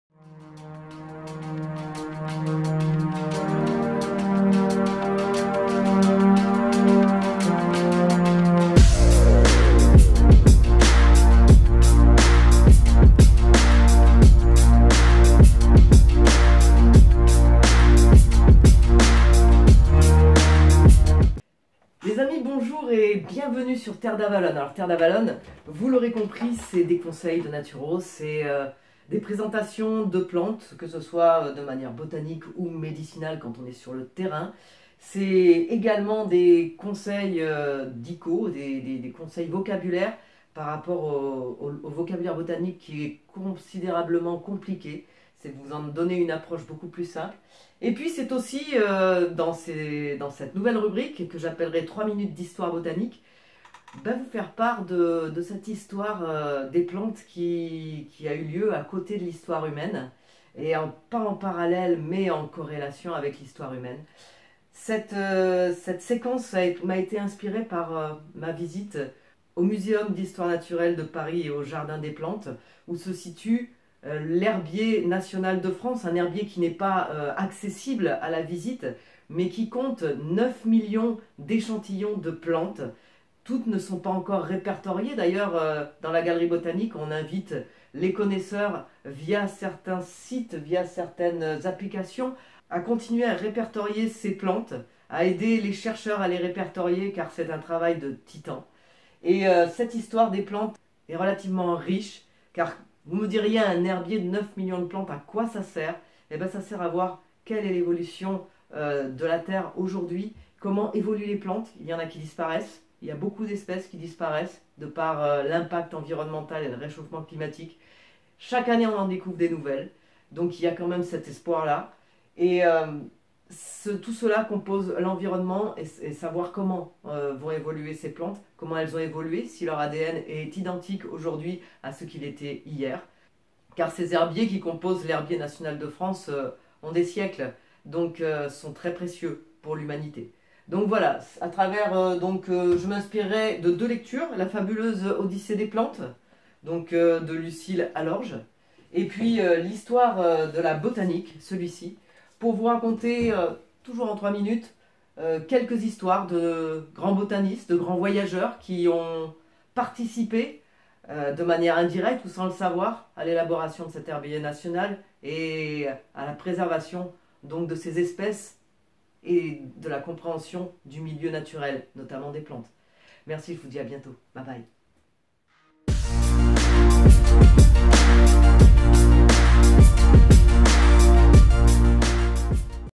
Histoire botanique en 3 minutes #1 : La préhistoire
L'histoire des plantes, voilà un sujet passionnant ! En effet la connexion de l'homme à la nature ne s'est pas faite du jour au lendemain... Il a fallu des siècles, des millénaires même pour que l'un apprivoise l'autre et pour que le monde des plantes prenne soin du monde des hommes... C'est au cours de ces 3 minutes d'histoire botanique que je vous propose de voyager chaque mois à travers le temps, le remonter jusqu'à nos origines puis commencer nos escales mensuelles, pour comprendre notre relation profonde à Dame nature et à nous même d'ailleurs!
Et cette semaine, notre premier arrêt se fait à la préhistoire ! N'hésitez pas à nous suivre sur Instagram, pour avoir du contenu supplémentaire !
La saison d'été touche à sa fin et la semaine prochaine, petit point naturo sur les poux ! Alors bonne semaine à tous, prenez soin de vous et surtout, restez connectés !

Livres cités :
- "Histoire de la Botanique" de Joëlle Magnin-Gonze
- "La Fabuleuse Odyssée des plantes" de Lucile Allorge